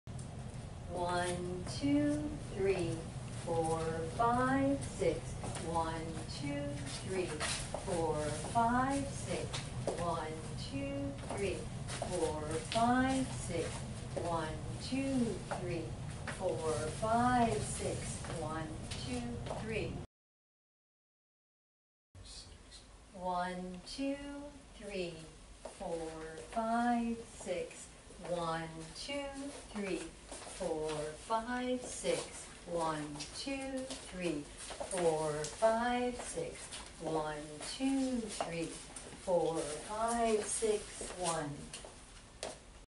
Waltz: Windshield Wiper
Reminiscent of stop and go in jive, I call it the windshield wiper because that's what the motion of the woman's held arm suggests to me..